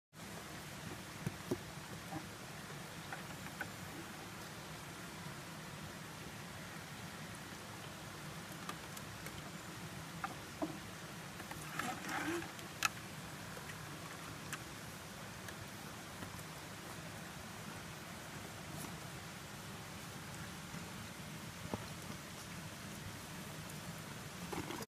Lavage
Katie Kobayashi (@katie_kobayashi) performing gastric lavage at Scott Creek (July 2017).